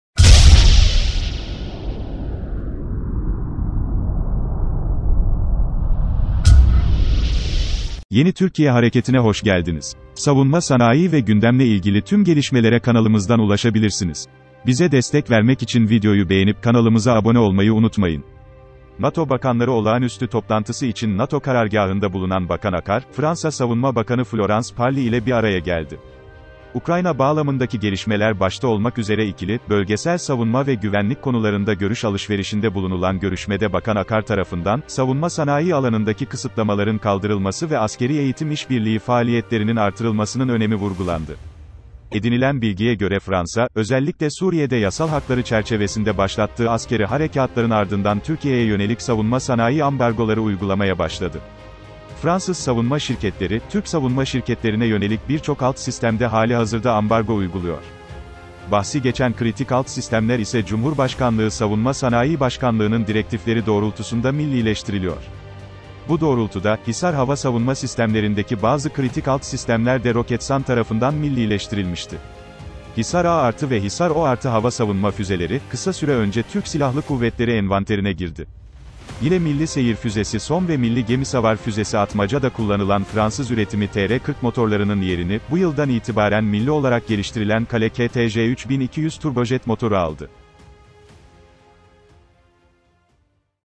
Bakan Hulusi Akar’dan Fransız Mevkidaşı Florence Parly'e Ambargo Vurgusu
KISA HABER METNİ :

NATO Bakanları Olağanüstü Toplantısı için NATO Karargahında bulunan Bakan Akar, Fransa Savunma Bakanı Florence Parly ile bir araya geldi. Ukrayna bağlamındaki gelişmeler başta olmak üzere ikili, bölgesel savunma ve güvenlik konularında görüş alışverişinde bulunulan görüşmede Bakan Akar tarafından, savunma sanayii alanındaki kısıtlamaların kaldırılması ve askerî eğitim iş birliği faaliyetlerinin artırılmasının önemi vurgulandı.

BİZ KİMİZ?